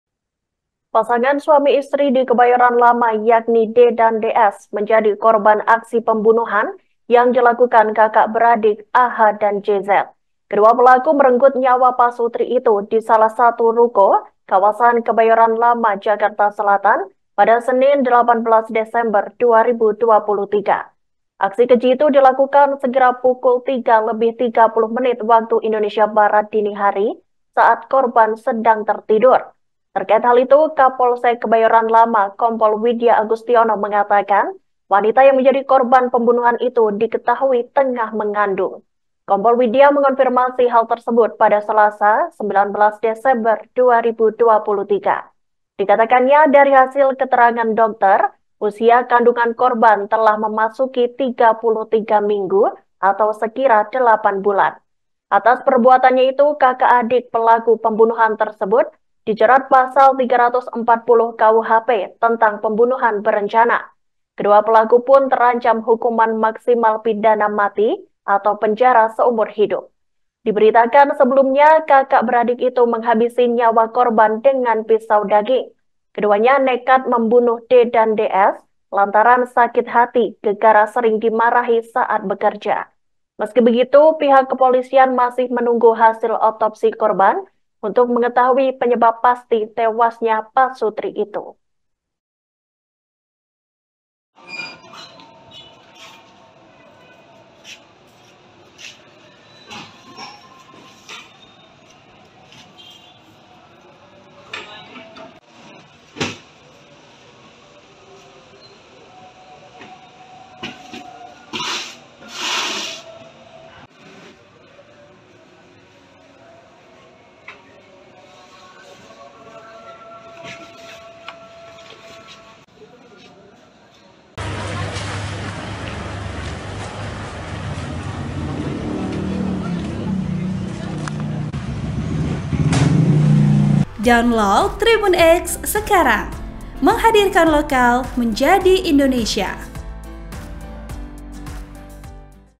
Korban Pembunuhan Kakak Beradik di Kebayoran Lama Ternyata Tengah Mengandung 8 Bulan
TRIBUN-VIDEO.COM - Pasangan suami istri di Kebayoran Lama, yakni D dan DS menjadi korban aksi pembunuhan yang dilakukan kakak-beradik, AH dan JZ.

Kedua pelaku merenggut nyawa pasutri itu di salah satu ruko Kawasan Kebayoran Lama, Jakarta Selatan pada Senin (8/12/2023).

Aksi keji itu dilakukan sekira pukul 03.30 WIB dini hari saat korban sedang tidur.

Terkait hal itu, Kapolsek Kebayoran Lama, Kompol Widya Agustiono mengatakan, wanita yang menjadi korban pembunuhan itu diketahui tengah mengandung.

Kompol Widya mengonfirmasi hal tersebut pada Selasa (19/12/2023).

Dikatakannya, dari hasil keterangan dokter, usia kandungan korban telah memasuki 33 minggu atau sekira delapan bulan.

Atas perbuatannnya itu, kakak adik pelaku pembunuhan tersebut dijerat Pasal 340 KUHP tentang pembunuhan berencana.

Kedua pelaku pun terancam hukuman maksimal pidana mati atau penjara seumur hidup.

Diberitakan sebelumnya, kakak beradik itu menghabisi nyawa korban dengan pisau daging.

Keduanya nekat membunuh D dan DS lantaran sakit hati gegara sering dimarahi saat bekerja.

Meski begitu, pihak kepolisian masih menunggu hasil otopsi korban untuk mengetahui penyebab pasti tewasnya pasutri itu.

(Tribun-Video.com/Iraka)

Artikel ini telah tayang di WartaKotalive.com dengan judul Wanita Korban Pembunuhan Kakak Beradik di Kebayoran Lama Sedang Hamil 8 Bulan,
Penulis: Nurmahadi | Editor: Dian Anditya Mutiara

Host: Iraka
VP: M Adnan